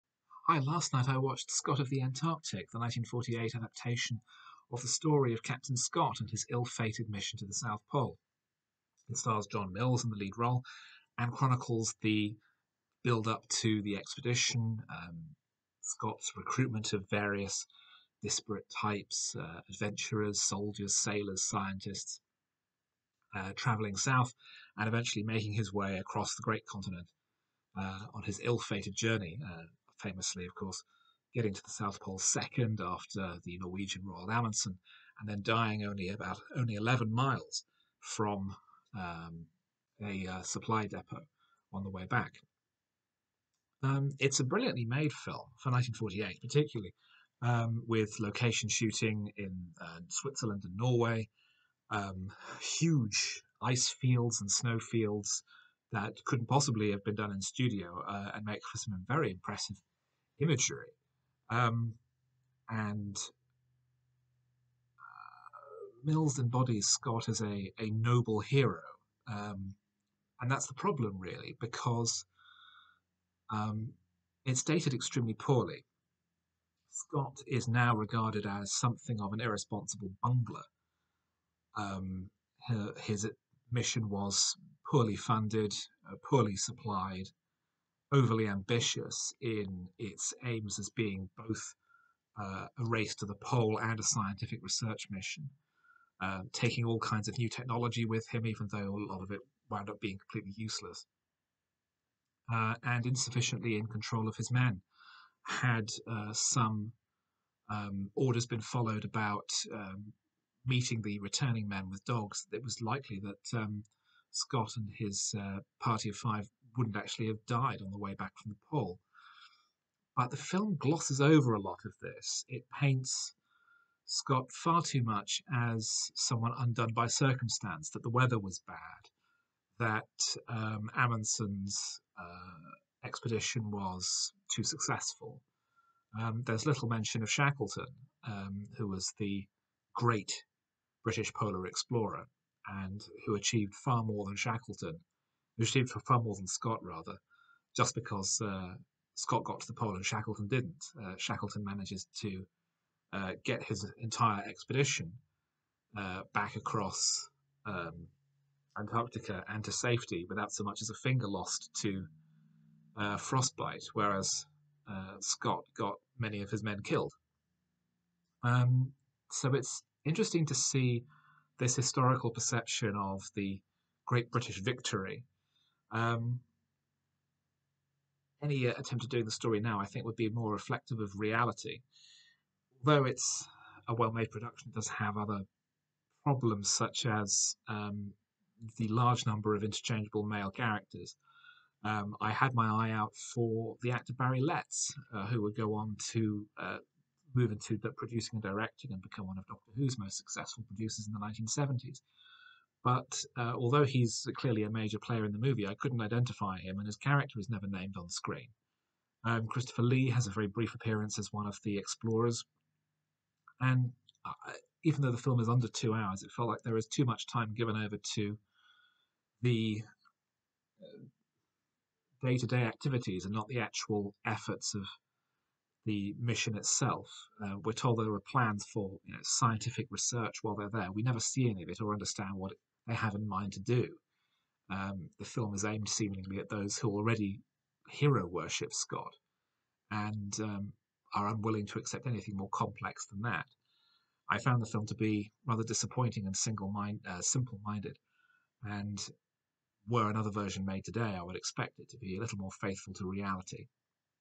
First Takes - Scott of the Antarctic (1948)
Jeremy watches the British historical adventure drama directed by Cahrles Frend and starring John Mills, Kenneth More, Harold Warrender and Reginald Beckwith.

Cinema Limbo is also a podcast.